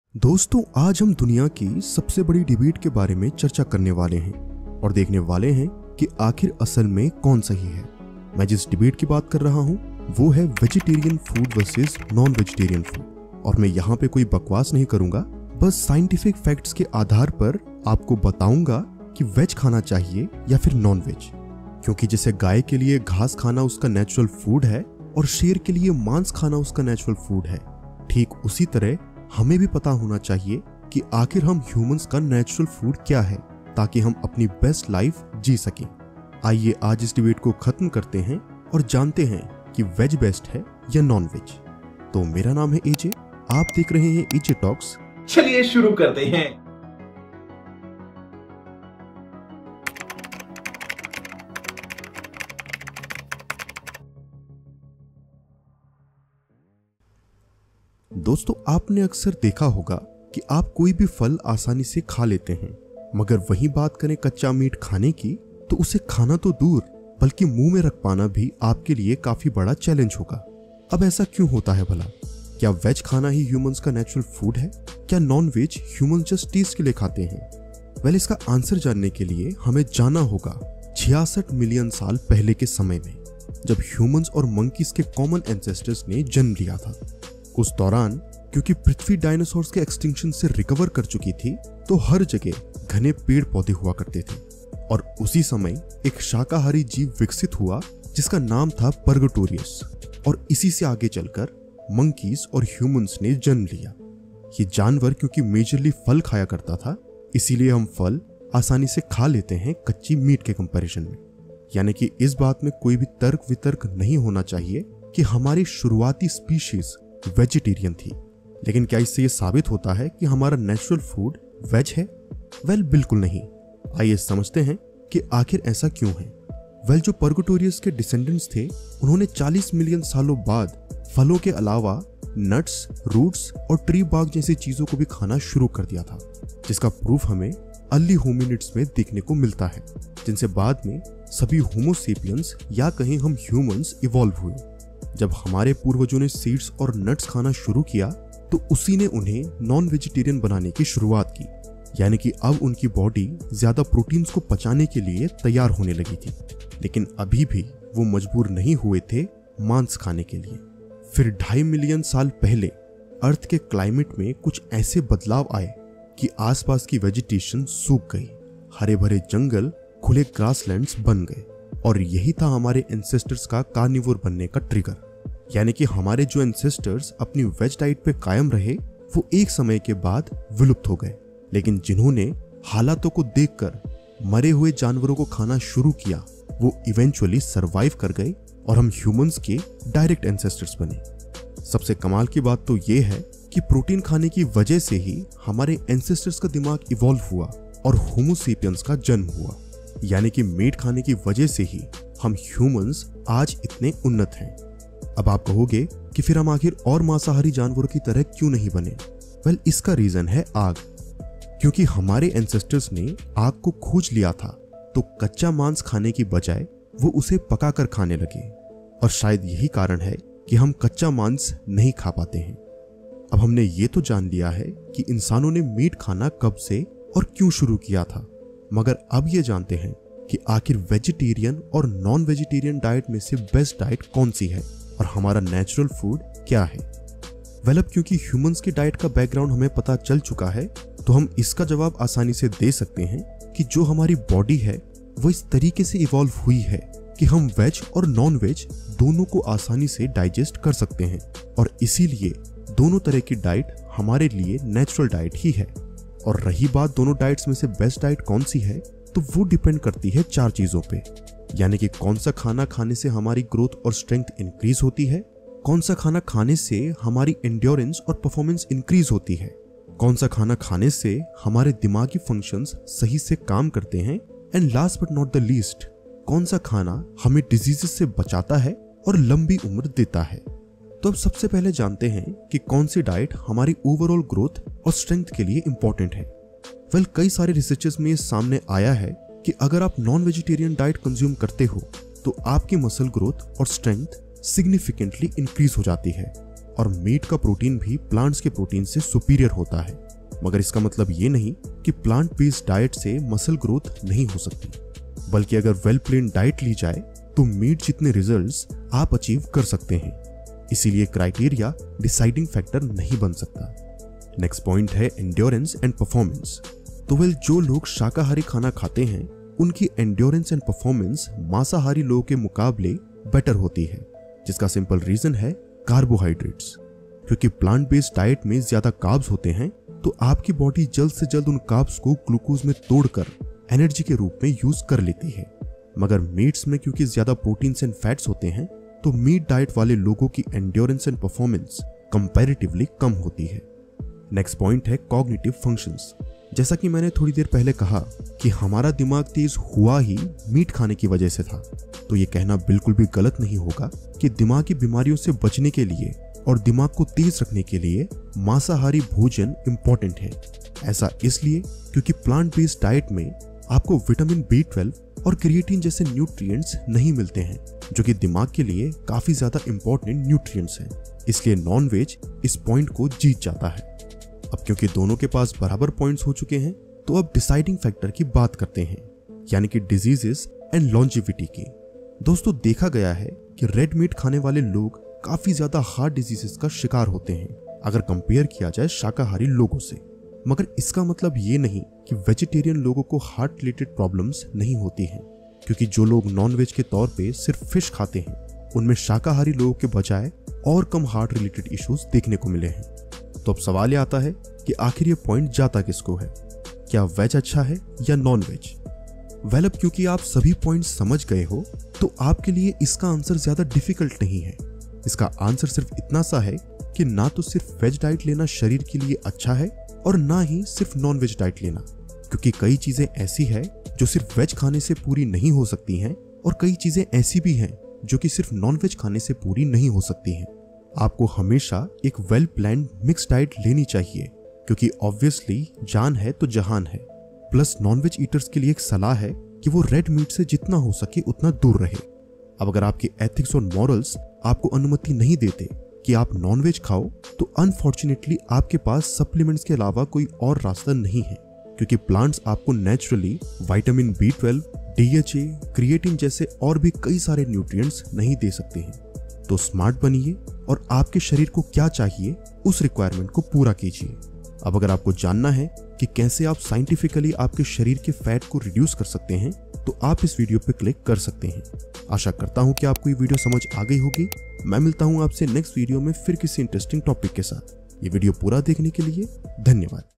Veg vs Non-Veg? Which is Better? | AJ-TALKS
🍎🥩 Veg vs Non-Veg? Which is Better? 🥩🍎

Friends, today we dive into one of the world's biggest debates: Vegetarian vs Non-Vegetarian Food! We'll explore which is truly better based on scientific facts.

Like how grass is natural for cows and meat for lions, we need to understand what our natural food is to live our best life. Join me, AJ, on AJ-TALKS as we end this debate once and for all!

Did you know that our early ancestors were predominantly fruit eaters, which is why fruits are easy to eat compared to raw meat? But does this mean vegetarian food is our natural diet? Let's travel back 66 million years to find out! 🦖🌿

Discover how our ancestors' diet evolved and what this means for us today. Whether you're a veggie lover or a meat enthusiast, you'll learn the benefits of both diets and which one might be best for you.

🔍 Watch to find out:

 1. Which diet boosts growth and strength? 💪
 2. Which diet enhances endurance and performance? 🏃‍♂️
 3. Which diet supports cognitive functions? 🧠
 4. Which diet helps prevent diseases and promotes longevity? 🩺

Ready to uncover the truth? Click now and let's get started!